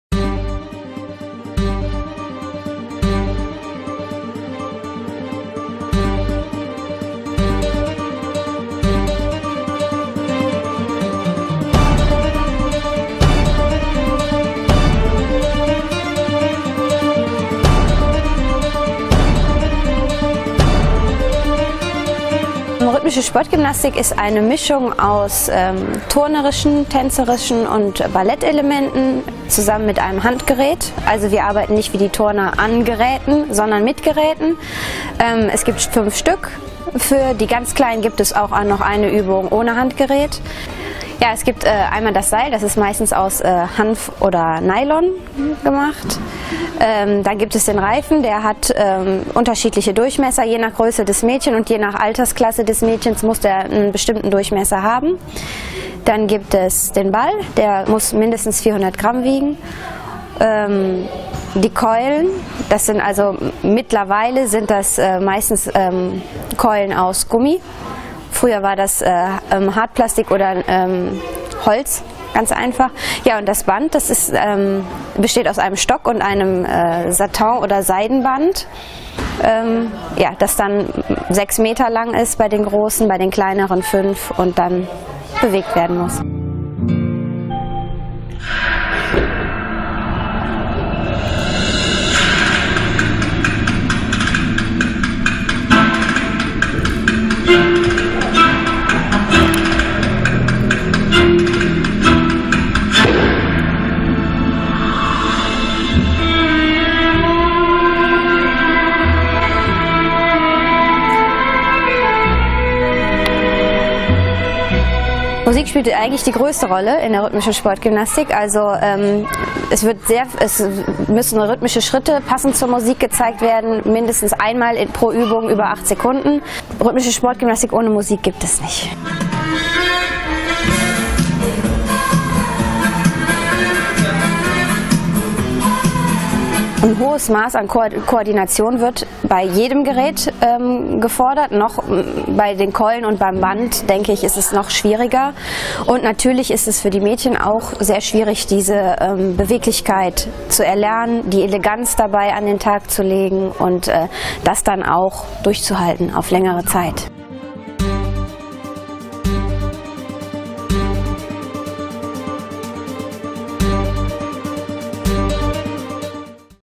Rhythmische Sportgymnastik - Akrobatik trifft auf Ästhetik
Bei der Rhythmischen Sportgymnastik treffen turnerische und tänzerische Elemente auf Bewegungen aus dem Ballett. Bei den Choreografien werden zudem zusätzlich verschiedene Handgeräte wie Ball, Reifen oder Keulen verwendet. Abgestimmt auf ein im Vorfeld ausgewähltes Musikstück werden die Bewegungen dessen Rhythmus angepasst und sollten möglichst fließend und fehlerfrei ausgeführt werden.